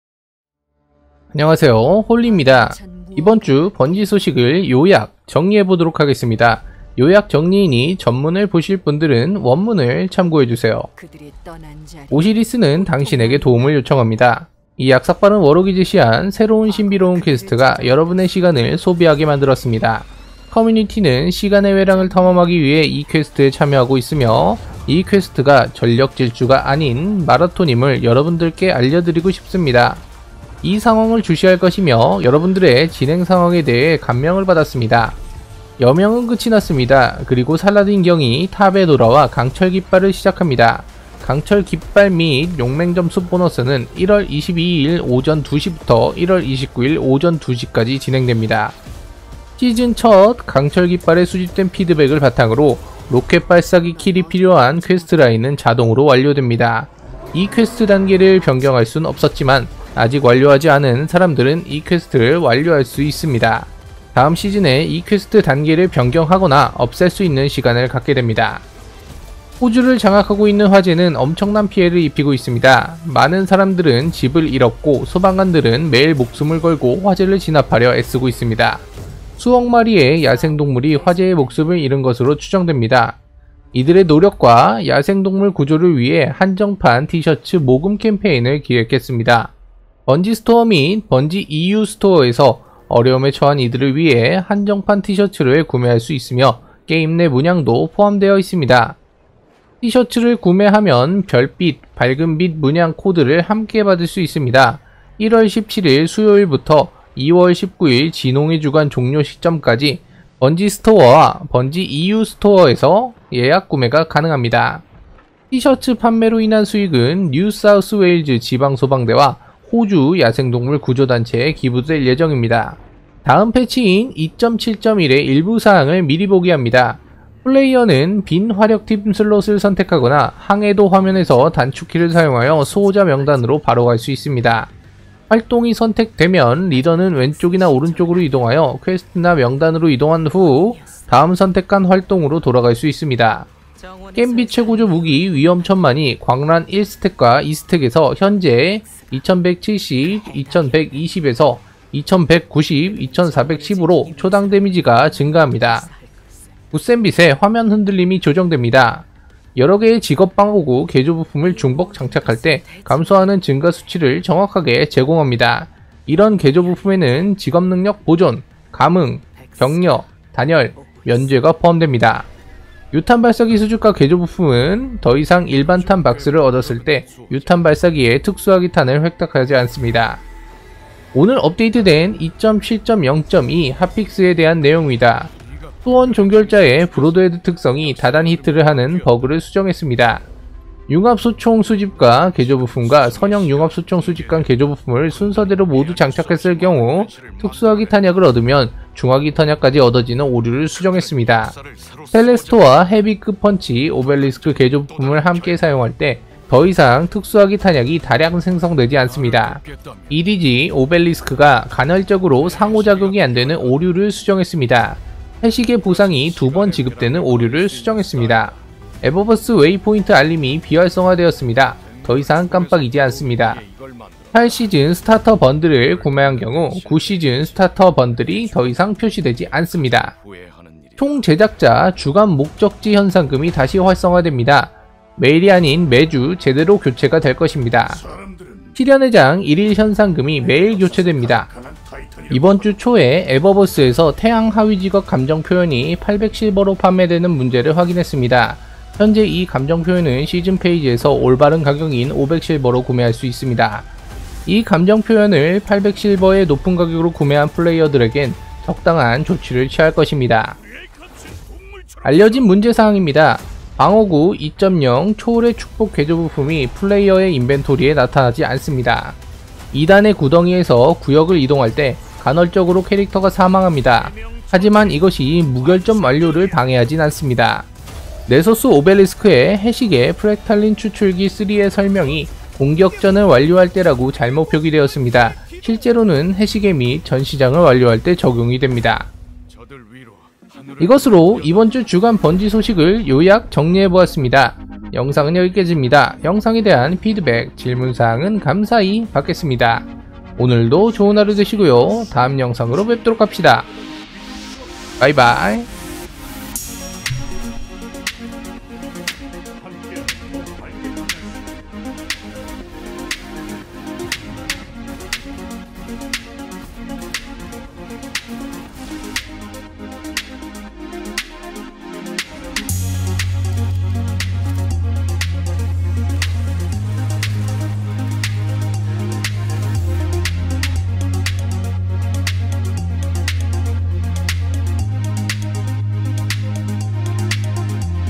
2020.1.17 이번주 번지 소식 EDZ 오벨리스크 소원종결자 픽스 위험천만 상향 강철깃발 개최 (Destiny 2) (데스티니 가디언즈 섀도우킵 서광의 시즌) [[VG]홀리]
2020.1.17 이번주 번지 소식 EDZ 오벨리스크 소원종결자 픽스 위험천만 상향 강철깃발 개최  (Destiny 2) (데스티니 가디언즈 섀도우킵 서광의 시즌) [[VG]홀리] 유튜브 자체자막 사용하고있습니다 자막이 필요하시다면 하단 자막(C)꼭! 꼭! 눌러주세요.